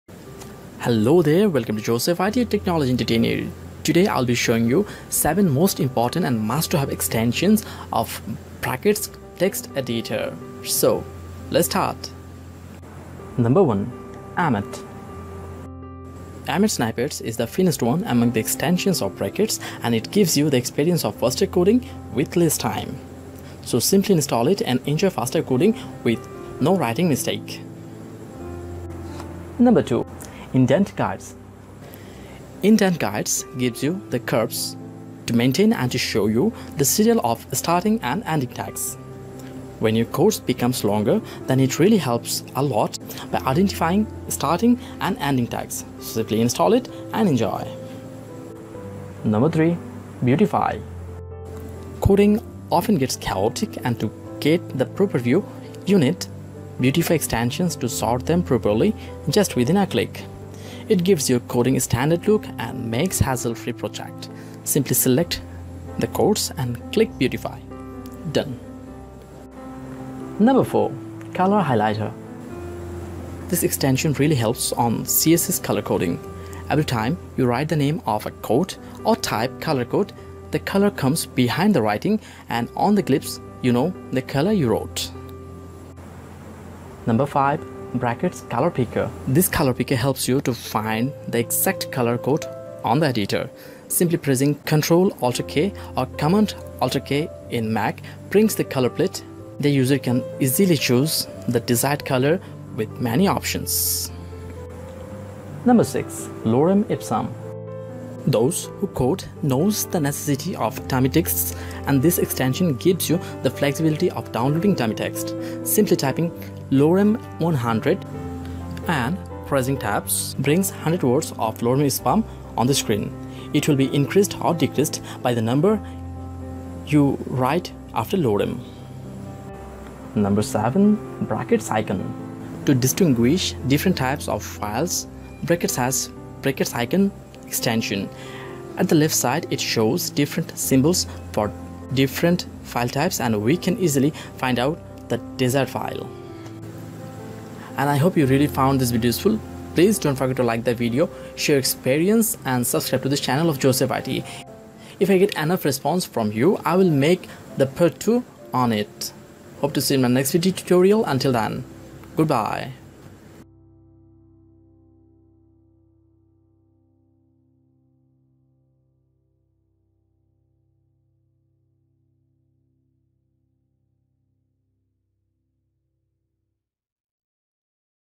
Brackets Code editor | Top 7 Extensions for better Coding Experience
Brackets Code edior is a Open source Text editor software and it really works well with better extensions or plugins. This video tutorials of Joseph IT shows the way to get top 7 extensions for better coding experience in Brackets Code editor.

There are thousands of Brackets Code Editor extensions and from them, we need some important or must to have!!! In this video tutorial, Joseph IT tried to introduce the most important extensions easily and quickly to make the viewers more benefited!!!

The seven extensions I discussed here are given below:

1.Emmet,
2.Indent Guides,
3. Beautify,
4.Color Highlighter,
5.Brackets Color Picker,
6.Lorem Ipsum,
7.Brackets Icon.

Stated extensions are really amazing and will really help the brackets user a lot to get their works done easily.

If I get enough response I will make part 2 on this topic and will bring more useful and important extension before you all. So , please let me know if you want it !

Share your experiences on the comment box and let us know about your feelings,